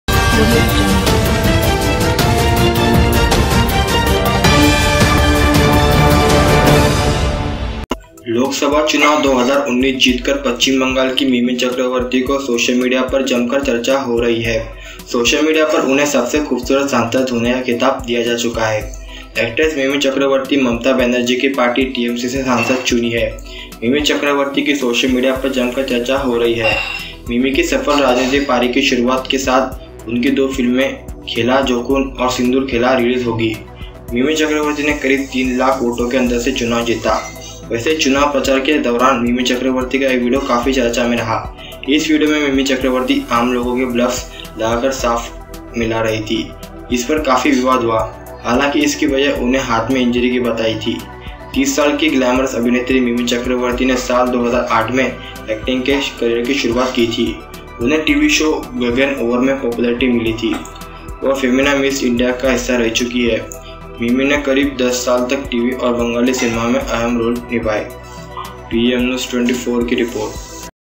ये हैं 17वीं लोकसभा की सबसे ग्लैमरस सांसद, क्रेजी है सोशल मीडिया
Winning the Lok Sabha election 2019, Mr. Mike Chakraborty of West Bengal's social media is getting a lot of discussion.
 The title of being a beautiful MP ​​has been given.
Actress mm Chakravarty Mamta Banerjee's party has been elected MP from TMC. Mimi Chakraborty's social media is being discussed very fiercely.
With the onset of successful political shift of Mimi this year, Jokes and Vines played in his two films this year will be played.
Mimi Chakraborty won the election by the difference of nearly three lakh votes. Well, during the election campaign, a video of Mimi Chakraborty was very much discussed.
 Was there. In this video, Mimi Chakraborti was mixing glyts with ordinary people. There was a lot of controversy on this. However, because of
 Injury was told in hand.
30-year-old glamorous actress, Mimi Chakraborty started acting career in 2008. Popularity from the TV show 'Ganer Opare'
 Was found.
She has been a part of Femina Miss India. Mimi has played important roles in TV and Bengali cinema for nearly ten years.